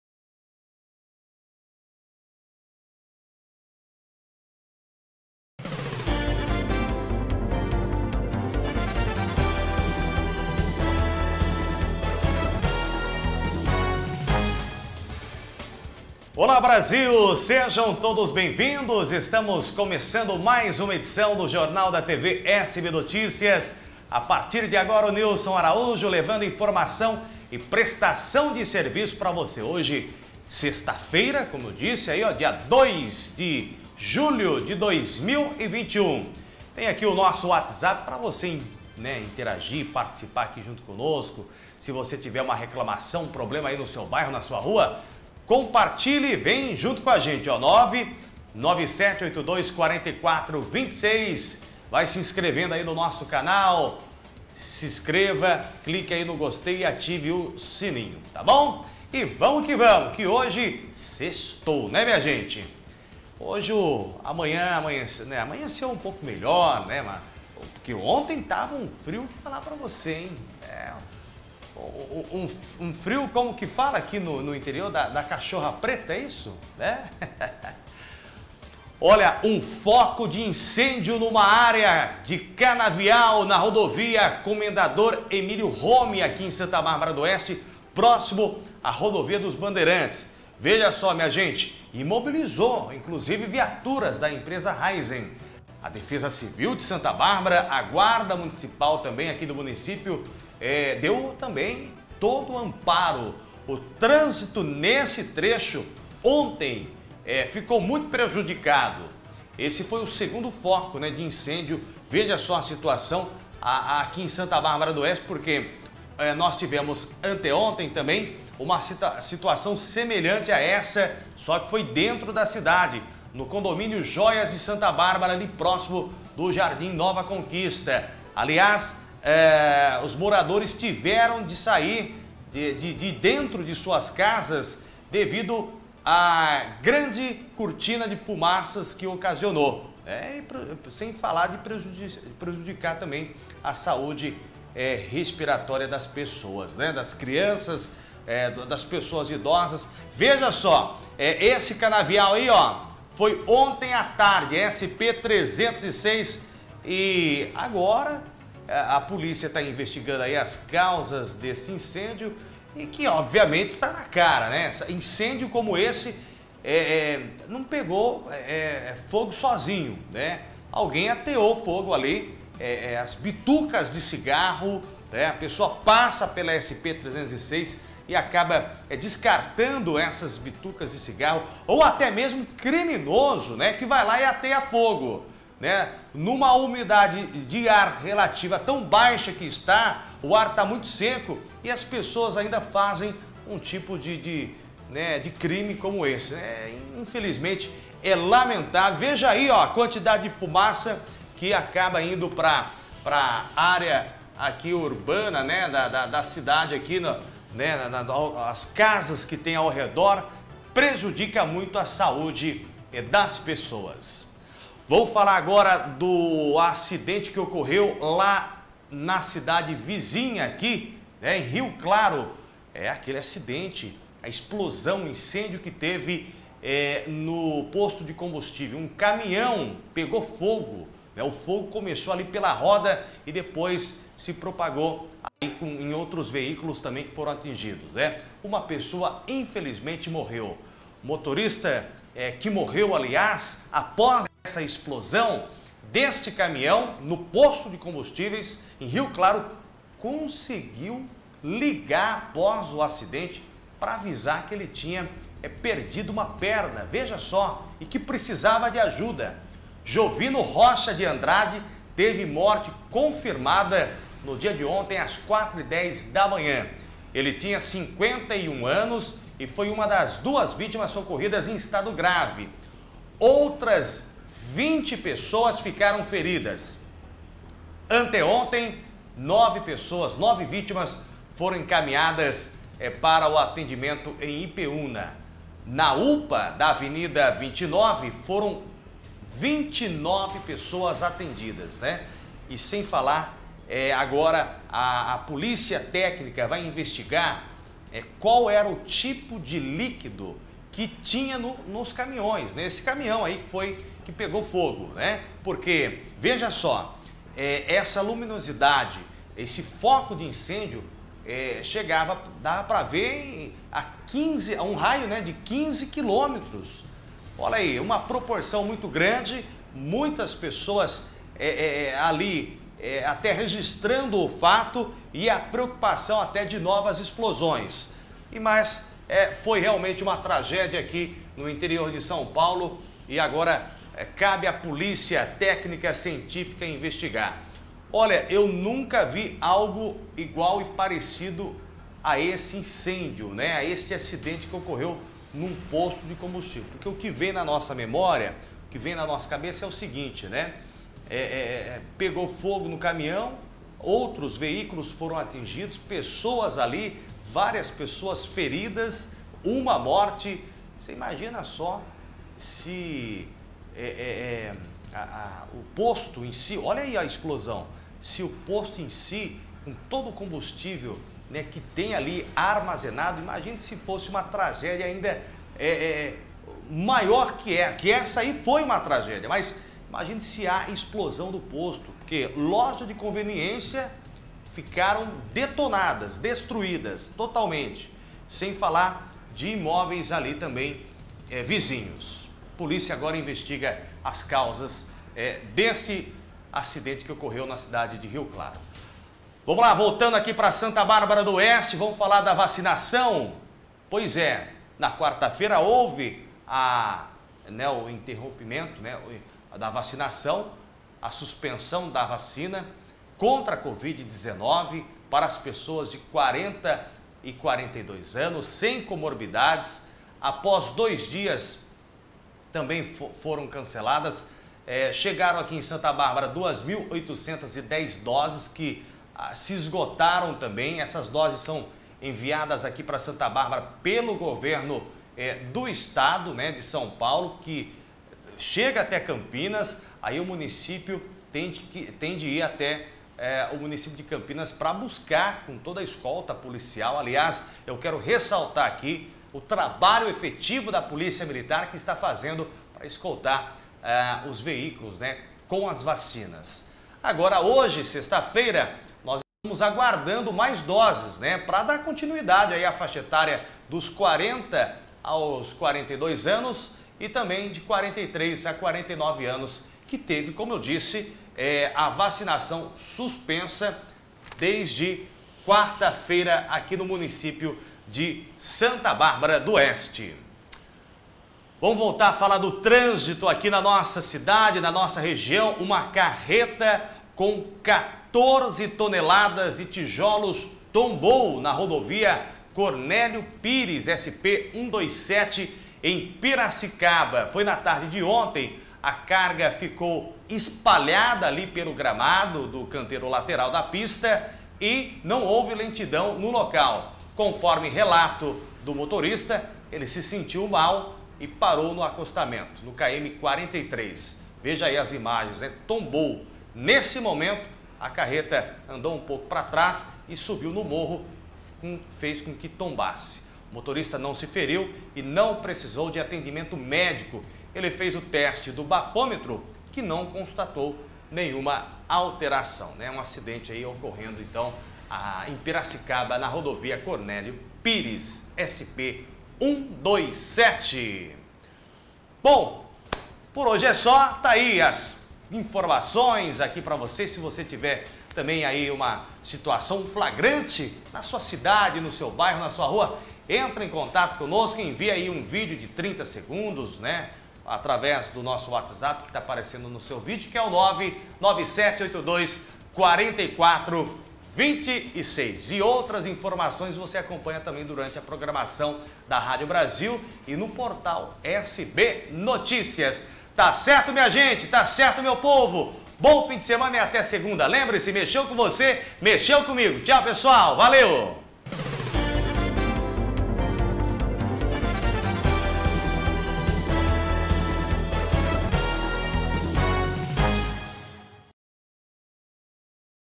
🔴🎬- JORNAL DA TV SBN – CARRETA COM 14 TONELADAS DE TIJOLOS TOMBA NA SP-127
Edição e produção: Josué Silva